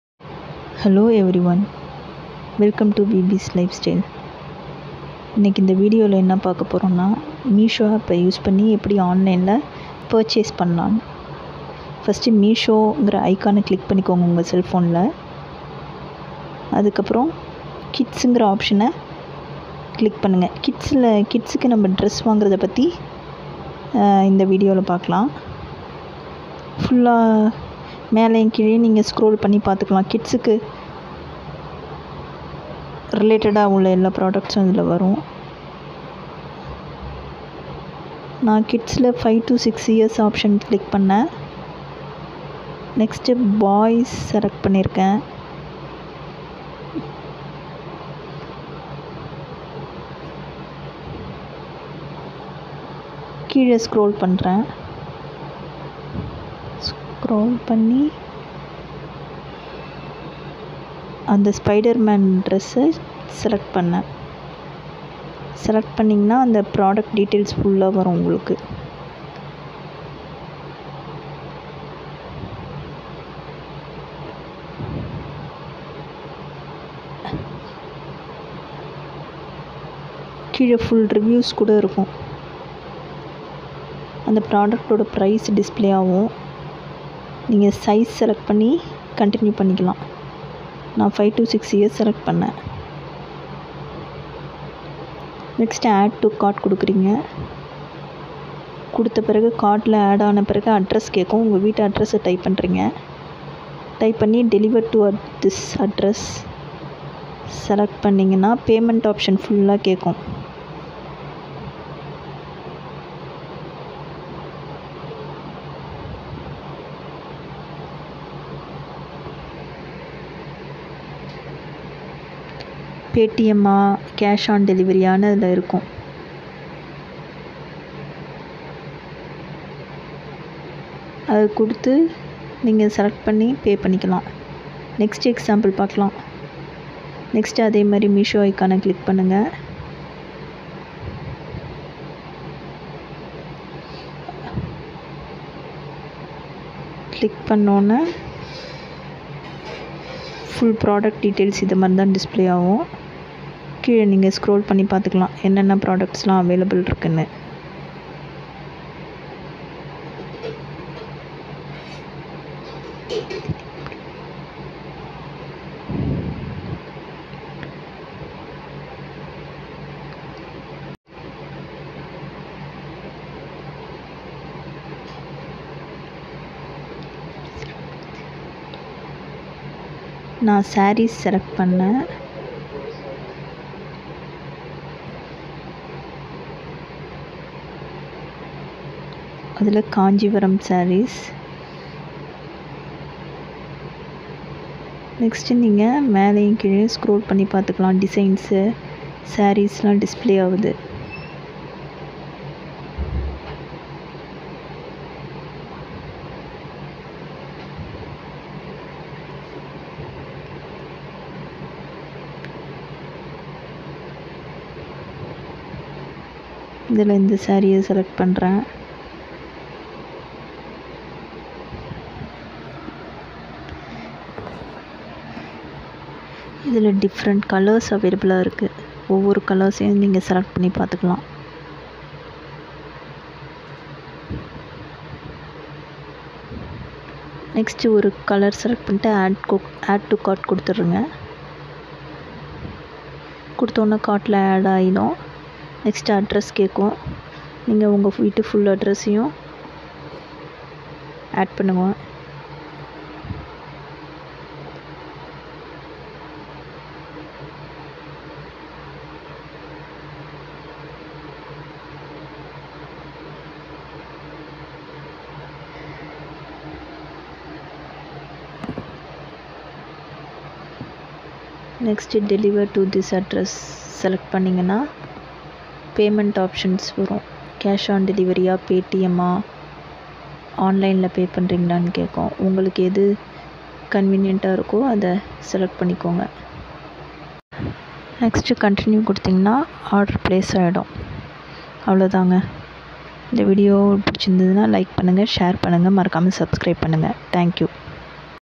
How to order in Meesho App
This video explains in detail how to order a product in Meesho App.